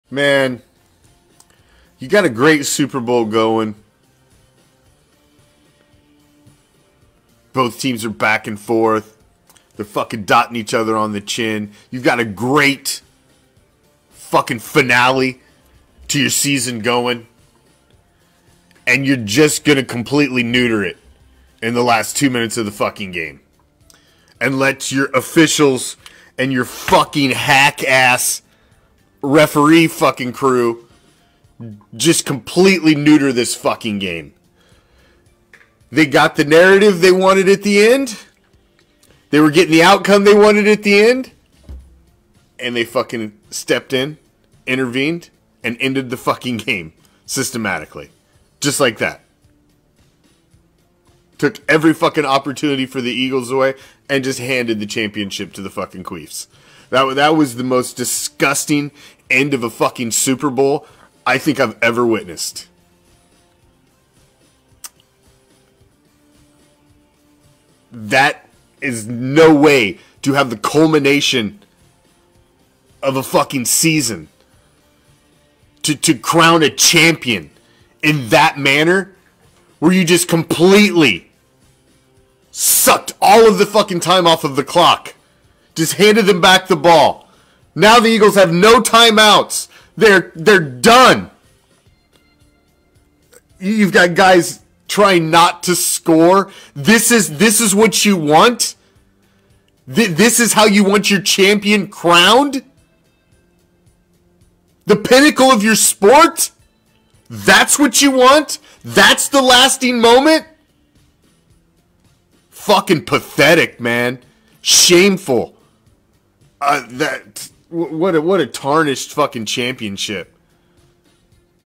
Super Bowl Postgame ReACTION
This Championship comes with an asterisk next to it. 🤦🏼‍♂️